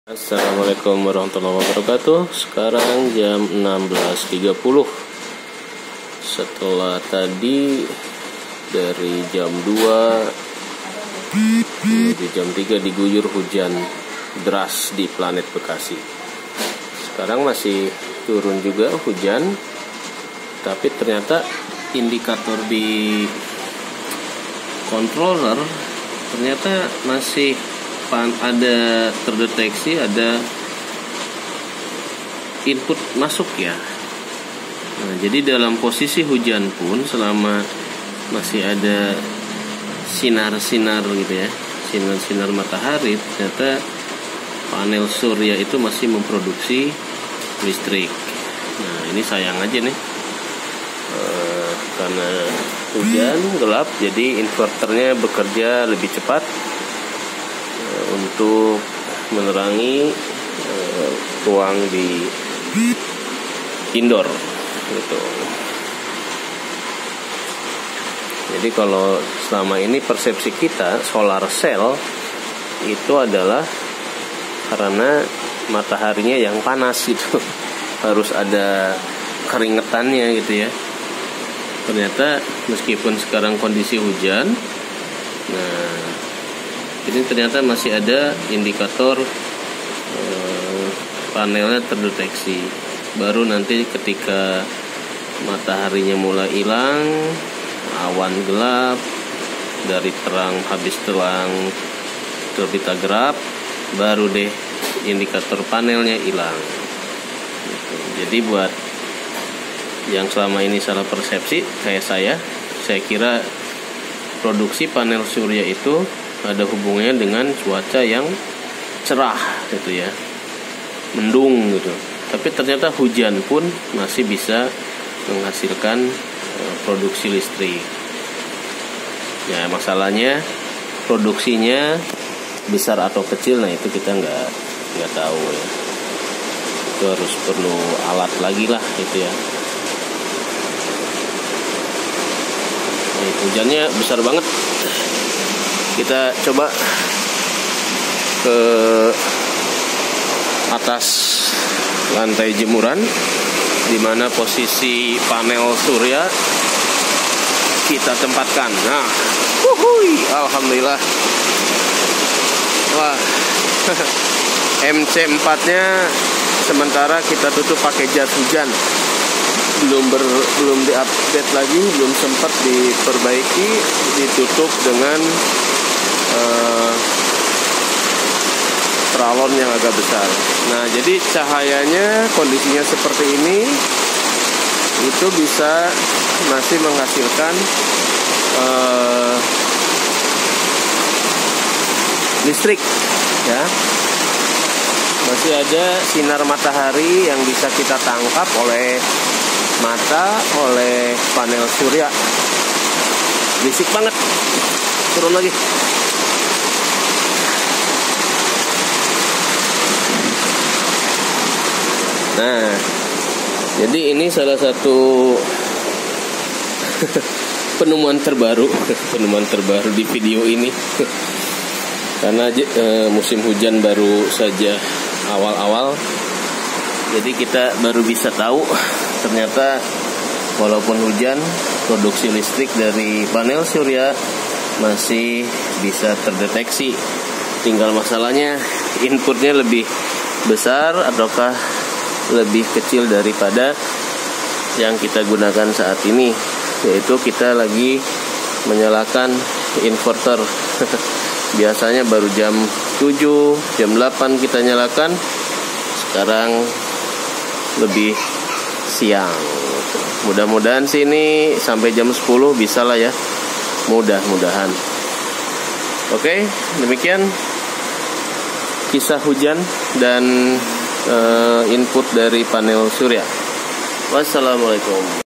Walau Hujan Besar... Panel Surya Tetap Memproduksi Listrik ternyata ya..?
Cuma. Arus listrik Yang Produksi nya entah brapa besar ...

# Musti dipindah ke bawah tiang lampu PLN nih panel surya nya !